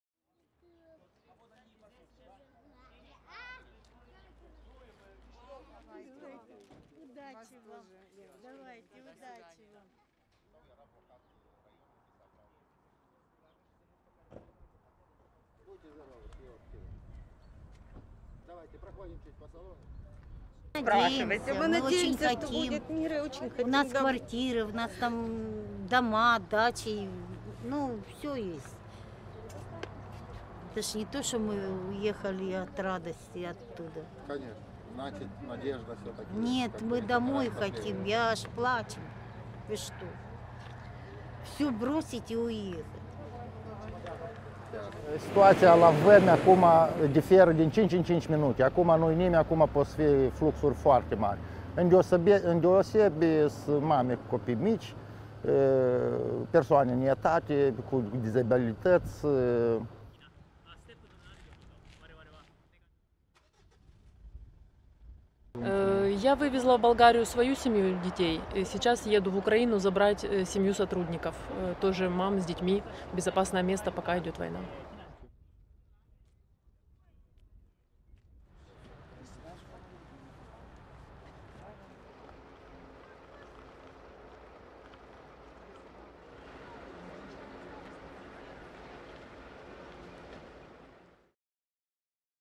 اللاجئون الأوكرانيون يواصلون التوافد إلى مولدوفا | AFP
يواصل مئات اللاجئين الأوكرانيين التدفق يوميا إلى مولدوفا، الدولة الصغيرة التي تجاهد للتعامل مع هذا التدفق. واستقبلت الجمهورية السوفياتية السابقة التي تعد 2,6 مليون نسمة نحو 300 ألف لاجئ، لا يزال نحو مئة ألف منهم على أراضيها.